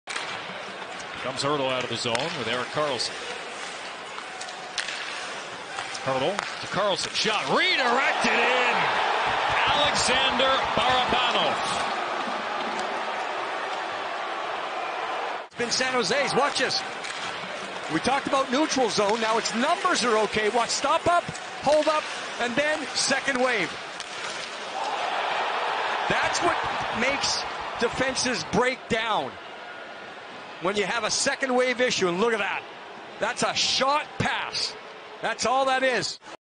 Александр Барабанов / Barabanov 12 гол в НХЛ 9 в сезоне (гол+пас 37) /11.03.2022/ (13326)
ВИДЕО № 1281

11.02.22 Александр Барабанов в матче против Лос-Анжелес Кингз забил 12 гол в регулярных чемпионатах НХЛ.  Шайба стала 9 в сезоне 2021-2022  Сан-Хосе Шаркс выиграл 4:3 ОТ

В составе «Сан-Хосе» шайбы забросили российский защитник Александр Барабанов, а также Брент Бёрнс, Ник Бонино и Томаш Гертл.

В составе «Лос-Анджелеса» точными бросками отметились форварды Филлип Дано, Андреас Атанасиу и Тревор Мур.